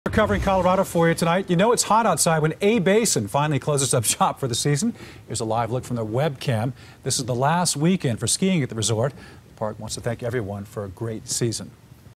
Last Weekend For Skiing At A Basin
The skiing is finally ending for the season at A Basin.